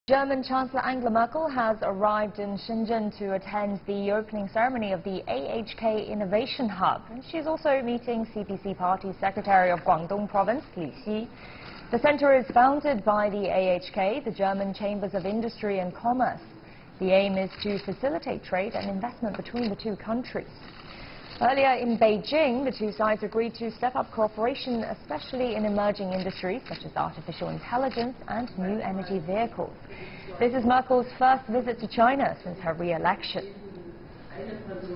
Merkel attends opening of AHK Innovation Hub in S China's Shenzhen
German Chancellor Angela Merkel has arrived in southern Chinese city of Shenzhen to attend the opening ceremony of the AHK Innovation Hub. She's also meeting with Li Xi, the CPC chief of Guangdong Province. The center, founded by the German Chambers of Industry and Commerce (AHK), aims at facilitating trade and investment between the two countries.